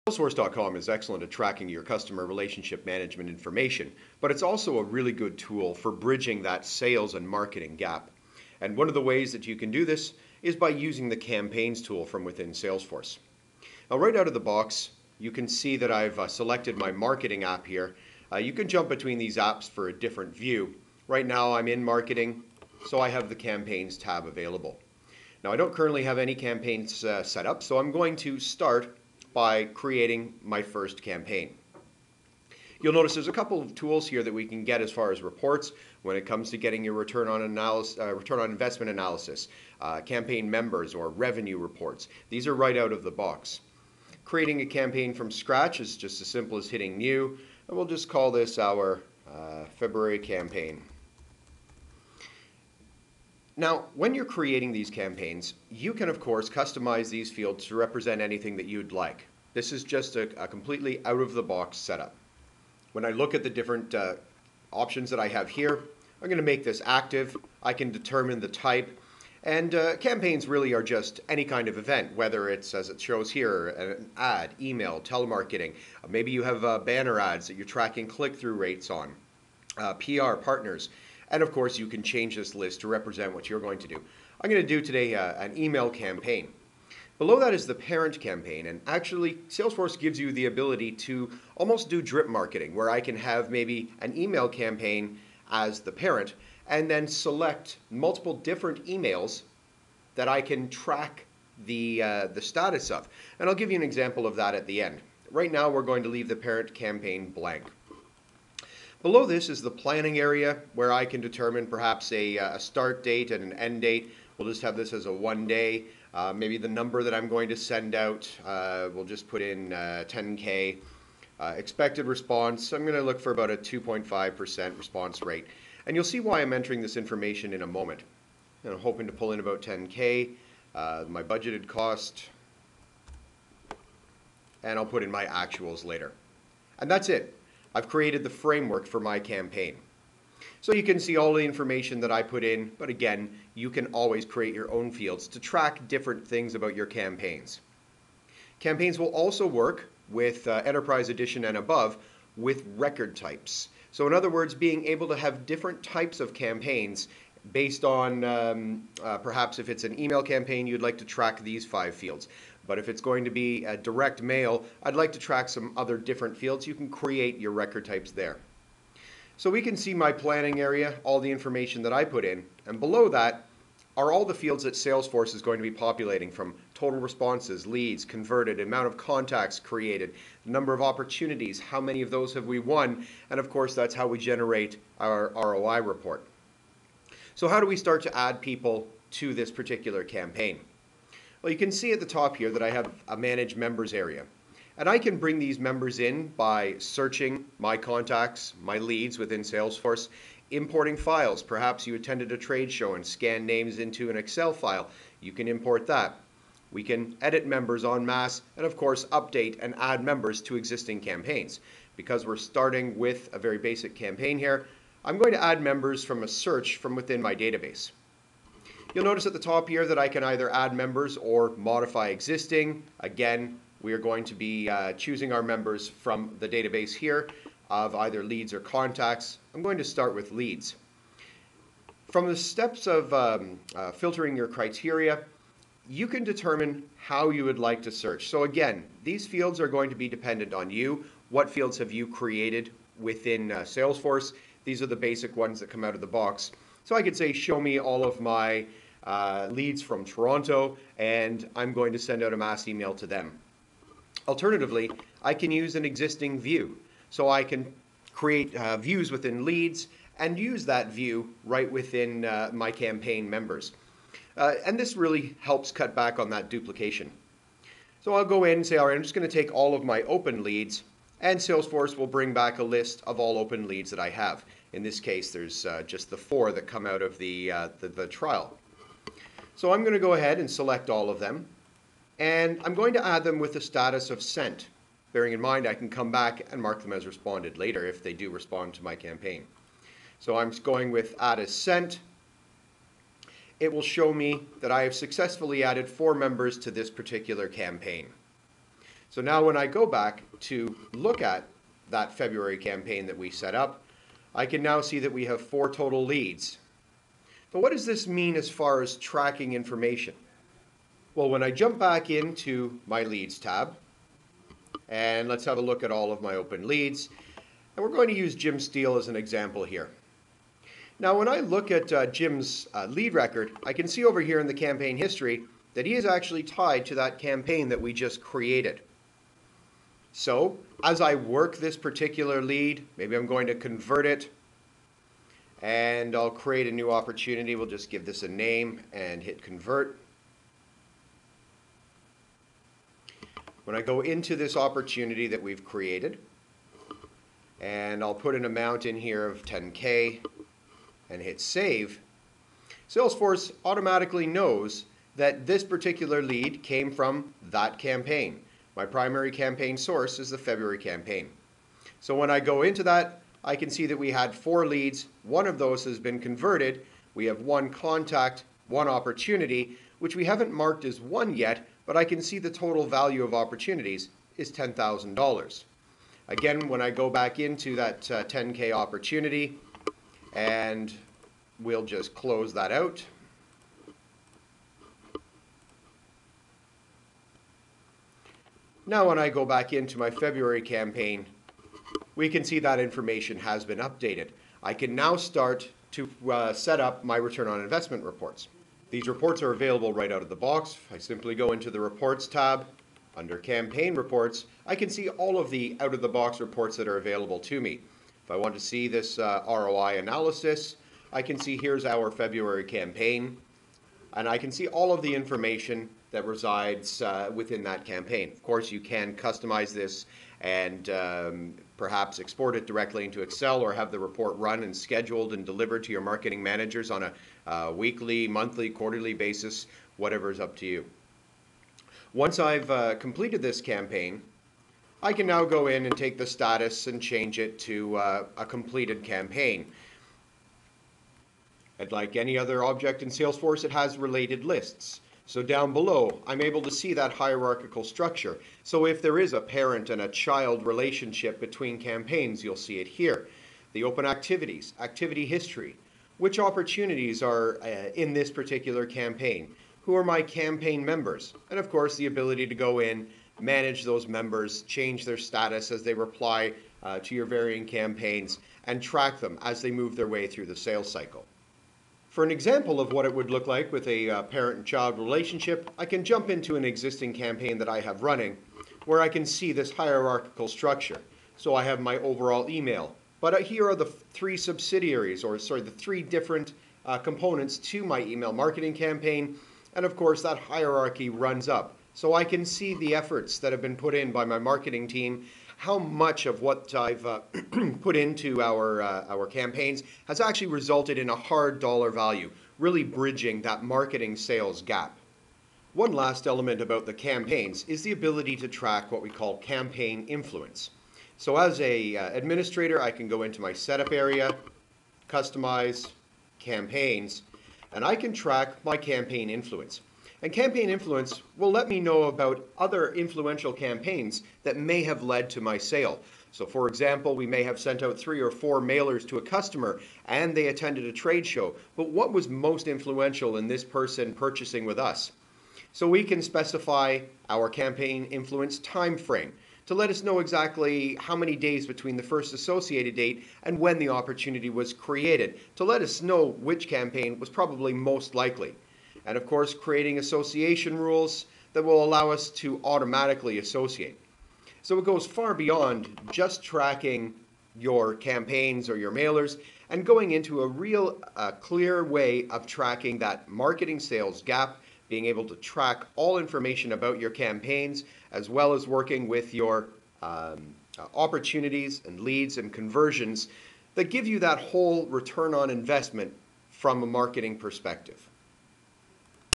Campaigns in Salesforce.com
How to set up a Campaign from scratch, add members, and track a ROI for a basica Campaign in Salesforce.  This is available for Professional Edition and above.